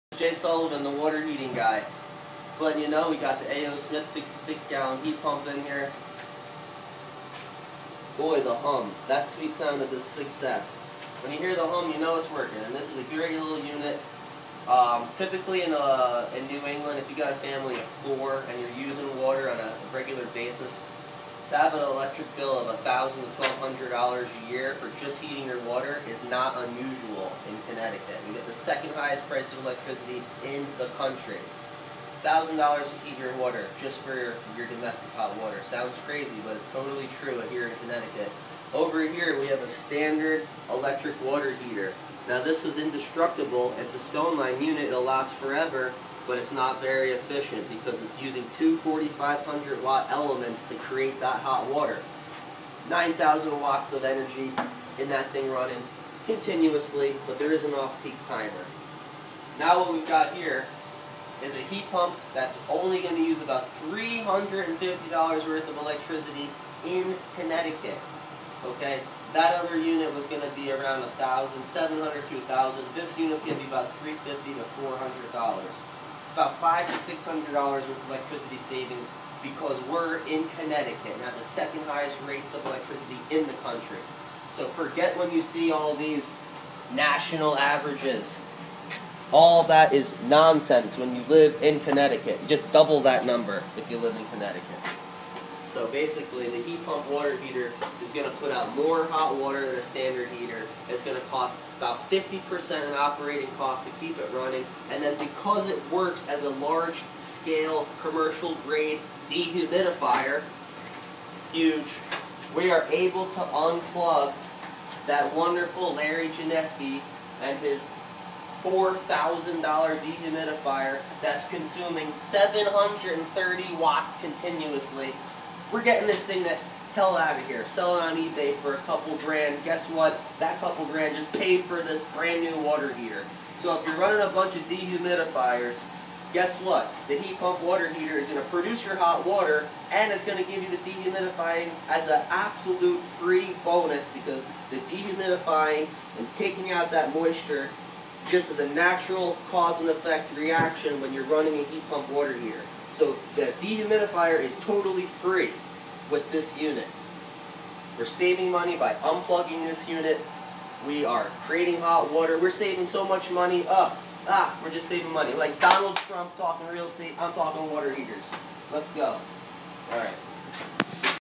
A.O Smith Heat Pump Voltex WHer
A.O. Smith 66 gallon Voltex electric hybrid heat pump water heater, Post installation review- and energy reduction overview.